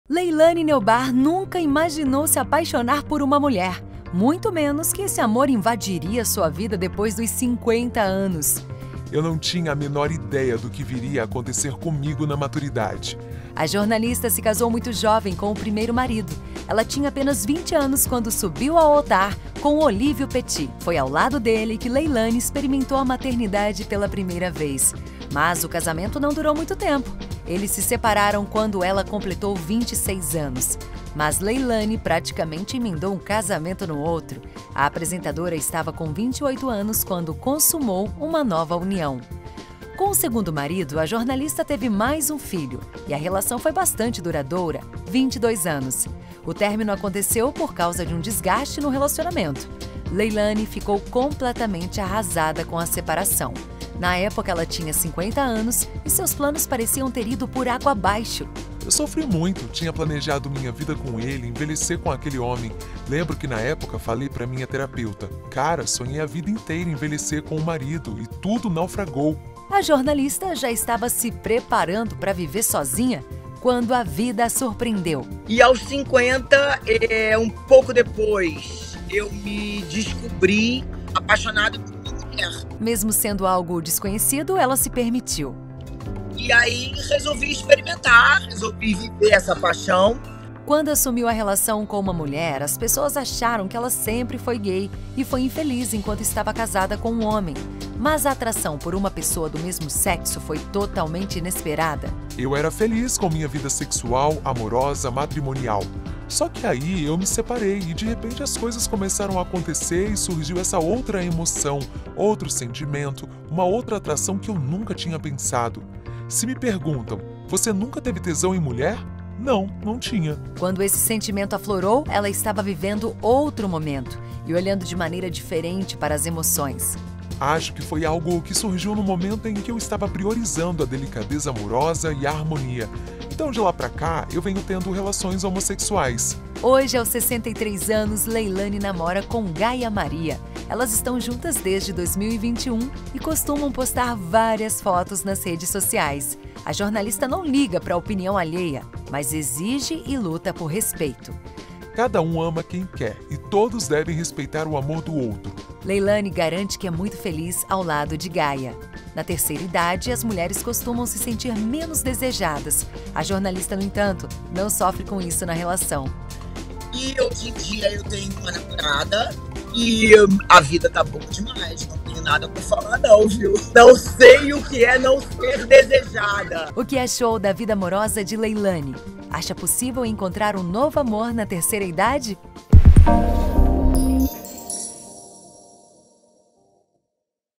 Leilane Neubarth se apaixonou por outra mulher aos 50 anos I Amor I VIX Brasil
A jornalista Leilane Neubarth nunca imaginou se apaixonar por outra mulher depois dos 50 anos. Conheça essa linda história!